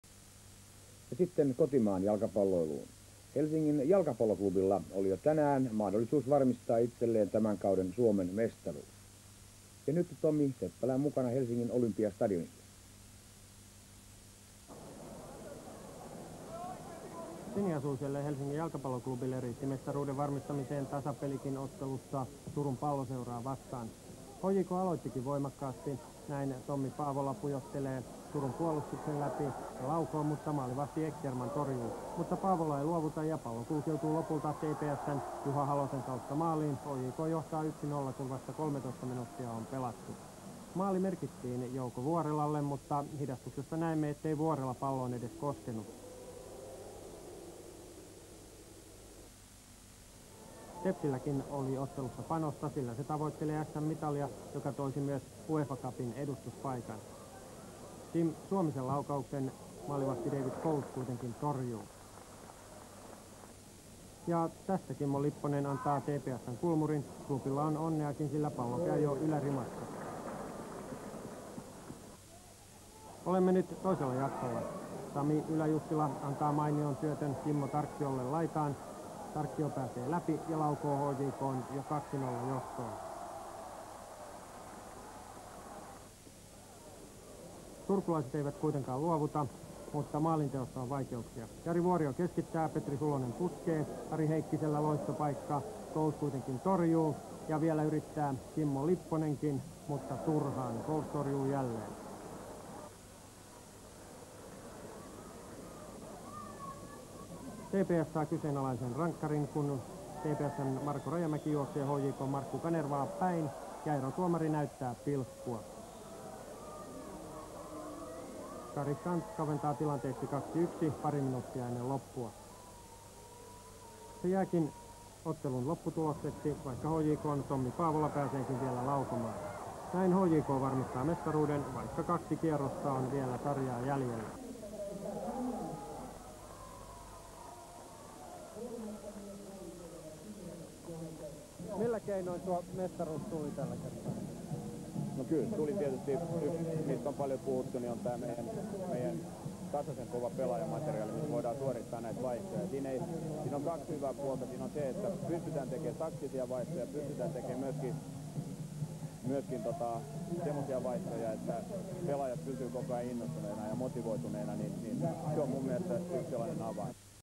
25.9.1988 | Mestaruussarja, yläloppusarja | HJK-TPS 2-1
1-0 Juha Halonen om (14)
1-1 Kari Skants (68/rp)
2-1 Kimmo Tarkkio (69)